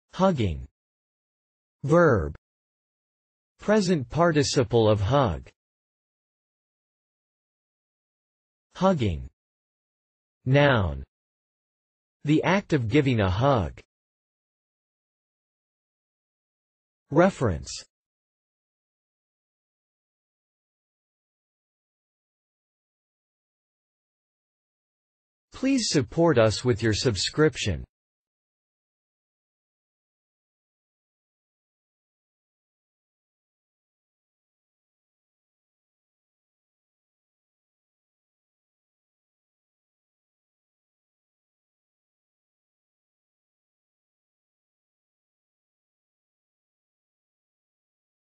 Hugging | Meaning of hugging 📖
See here, the meanings of the word hugging, as video and text.

hugging (verb)
    Present participle of hug.

hugging (noun)
    The act of giving a hug.

Reference:
    hugging